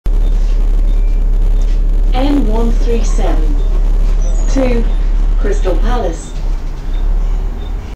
N137 to Crystal Palace
Route N137: Crystal Palace - Oxford Circus operated by Arriva London South for London Buses.
This route until 25th March 2012 terminates at Marble Arch.